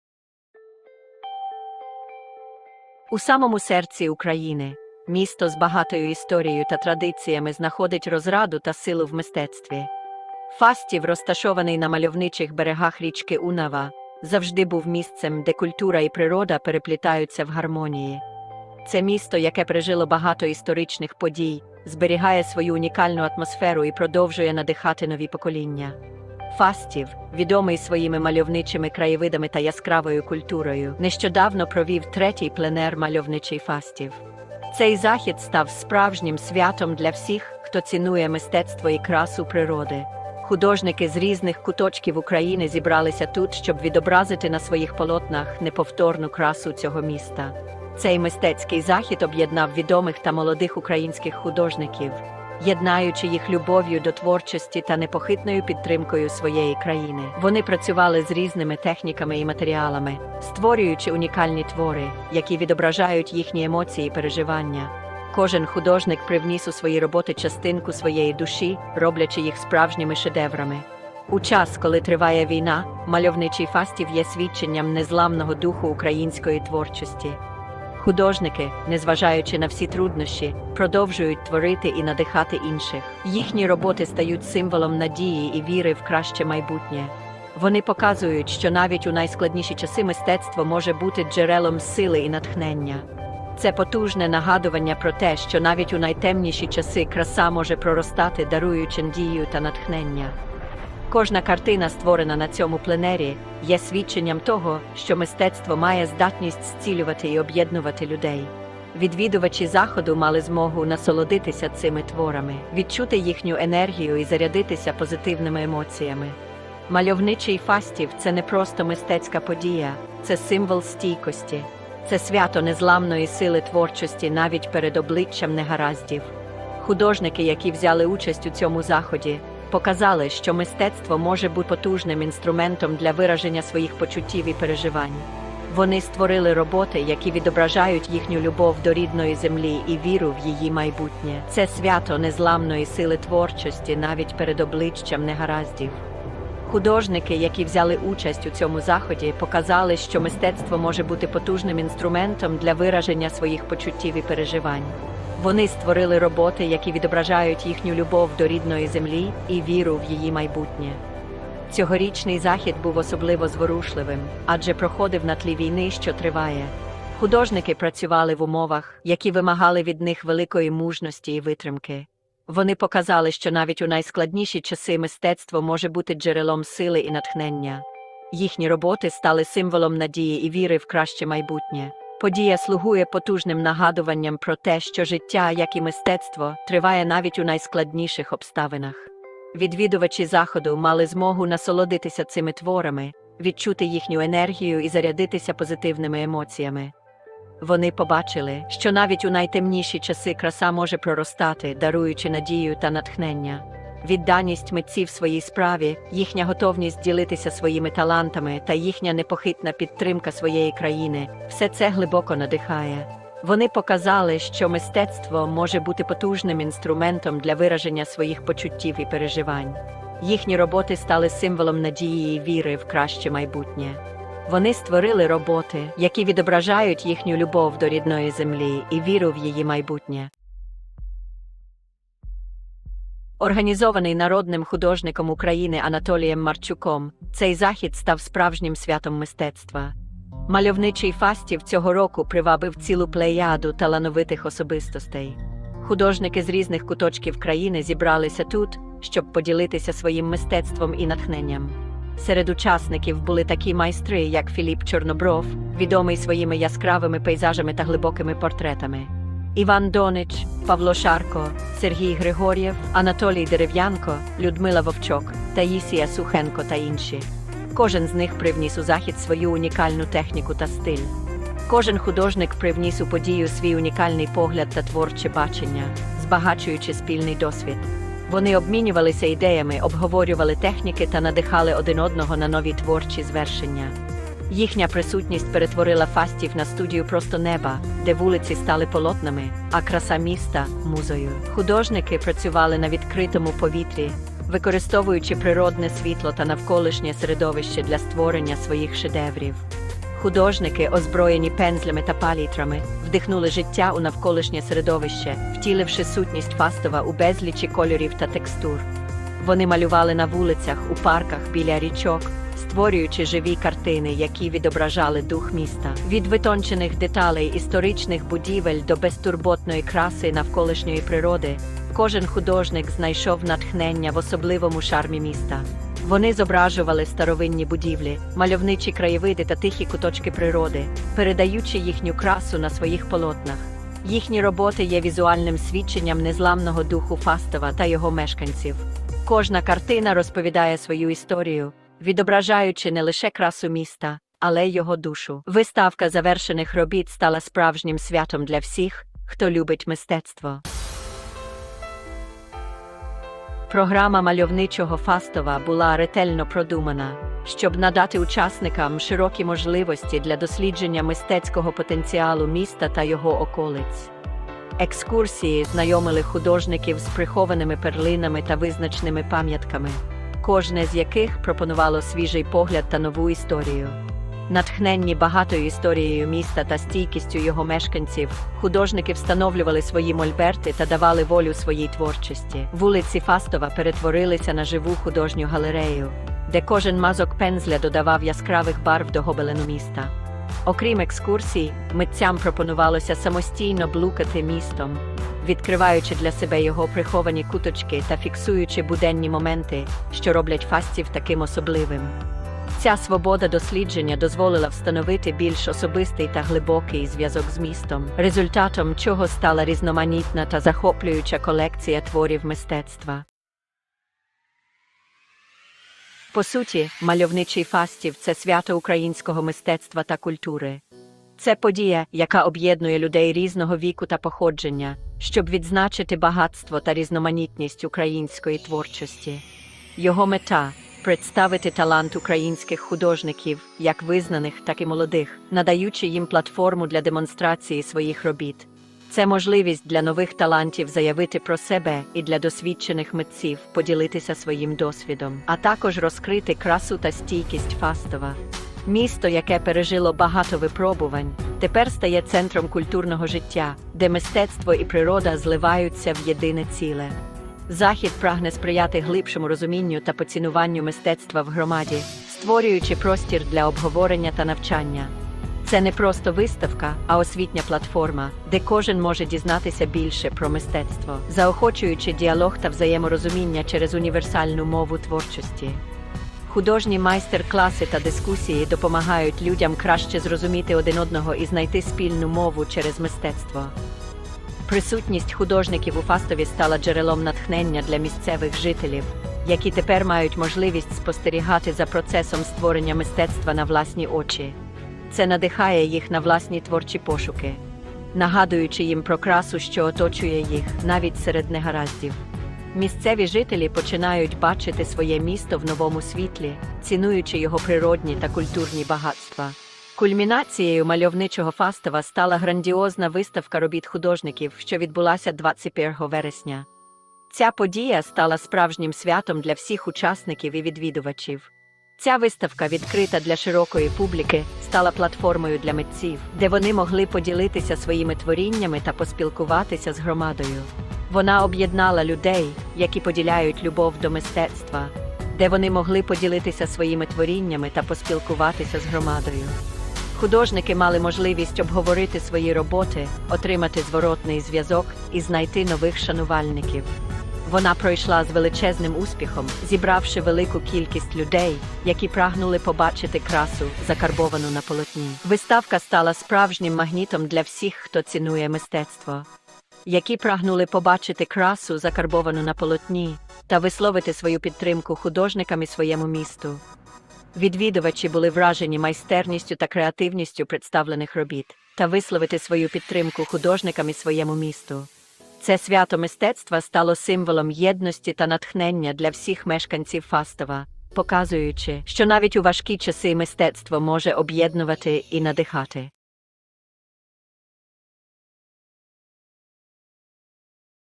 Фастів живописний: Третій пленер мистецтв у Фаст
OUTLINE:
00:00:00 A Brushstroke of Hope in Challenging Times
00:01:31 A Maestro Guides the Artistic Symphony
00:02:12 Inspiration Drawn from History and Nature
00:02:51 Weaving Tales Through Colors and Canvas
00:03:28 A Tapestry of Creativity and Resilience
00:04:08 Brushes United in Support of Ukrainian Defenders
00:05:58 Fostering Community and Inspiring Hope
00:07:20 A Beacon in Times of Adversity
00:08:05 Fascinating Fastiv - A Testament to the Power of Art

🌟 Вітаємо вас на каналі ФастівNews.City! 🚀 Сьогодні ми познайомимо вас з унікальною подією, яка проходить у нашому місті - третім пленером "Фастів живописний"! 🎉

📅 З 16 по 21 вересня 2024 року, художники з усієї України збираються у Фастові, щоб створити нові шедеври, надихаючись мальовничими краєвидами нашої громади! 🖌️✨

👨‍🎨 Організатором цього мистецького заходу є відомий Народний художник України, Анатолій Марчук, який вже третій рік поспіль об'єднує талановитих митців. Серед учасників - Філіп Чорнобров, Іван Донич, Павло Шарко та багато інших! 🌈

🌿 Під час пленеру художники матимуть можливість не лише малювати, а й проводити мистецькі прогулянки, відкриваючи для себе нові джерела натхнення! 🗺️🌸

🎨 Завершенням пленеру стане виставка, яка відбудеться 21 вересня, де ви зможете побачити результати творчої роботи художників! 💖

💰 Також у нас відбувається виставка-продаж картин, де частина коштів йде на підтримку ЗСУ. Підтримаймо українське мистецтво разом! 🇺🇦❤️